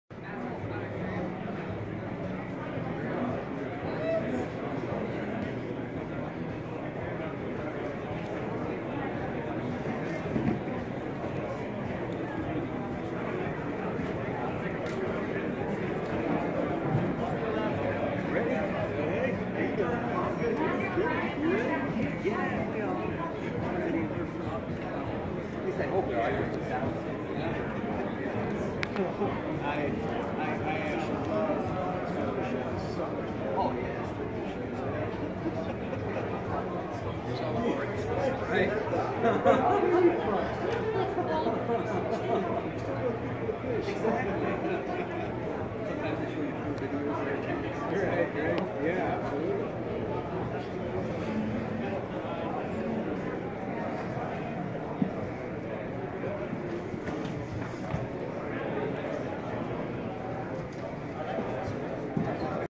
Synergy Reef CL34 Sump @ Reefapalooza Orlando 2017
One of our CL34 sumps on display at Reefapalooza Orlando 2017. We had it lit with the new Kessil H80 grow light. These LEDs are great for growing macro algae in a refugium.
This sump was etched on the front specifically for the Mr. Saltwater Tank giveaway. Standard sump will not have this etched into it.